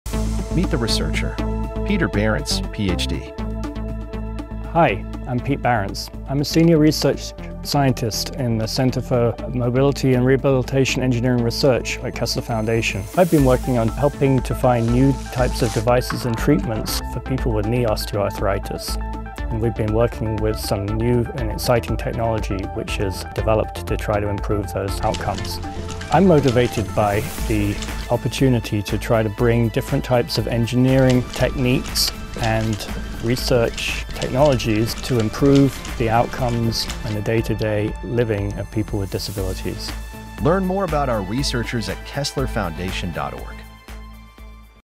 It's National Robotics Week!
Meet Peter Barrance, senior research scientist in the Center for Mobility and Rehabilitation Engineering Research at Kessler Foundation.